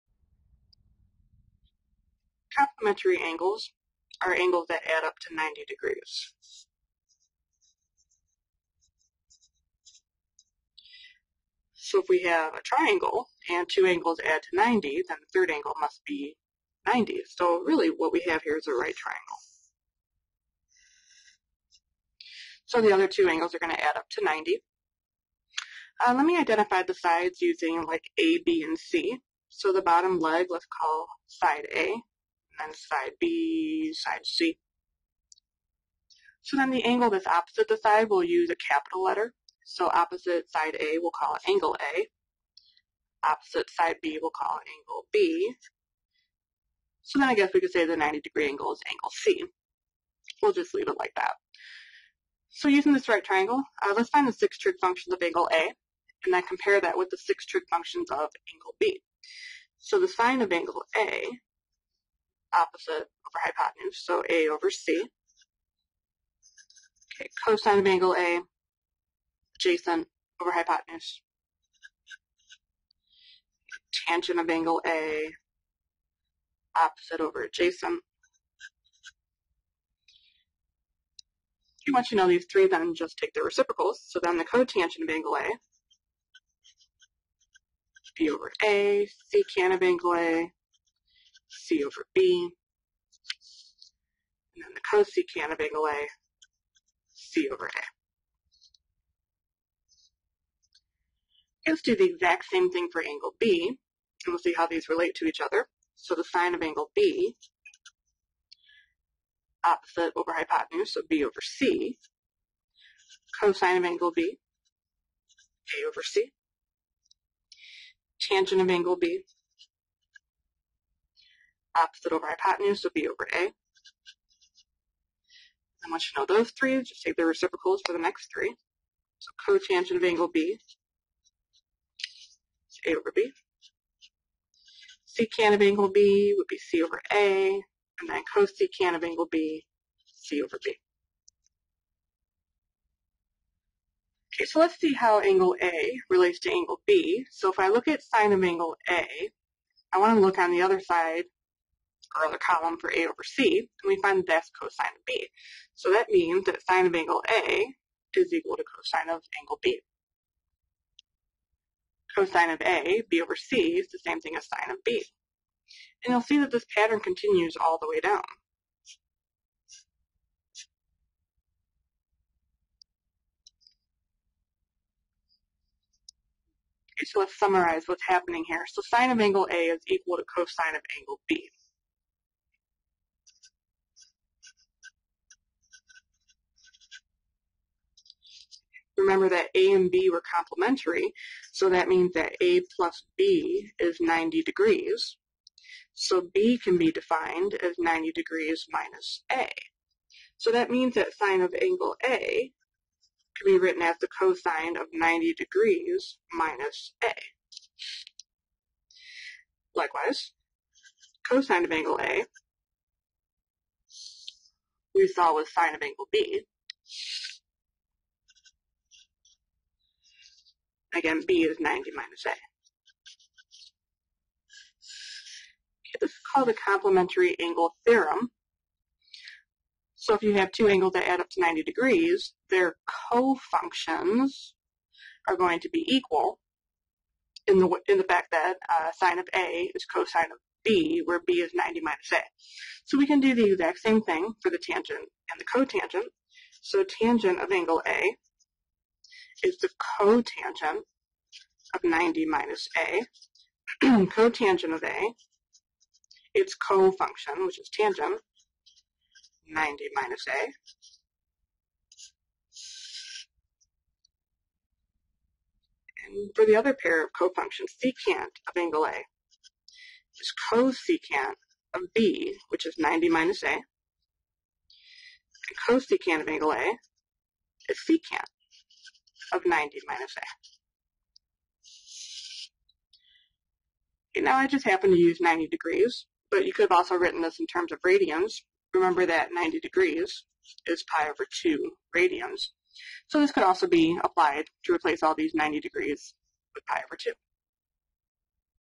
Complementary Angle Theorem
In this video, the Complementary Angle Theorem is derived.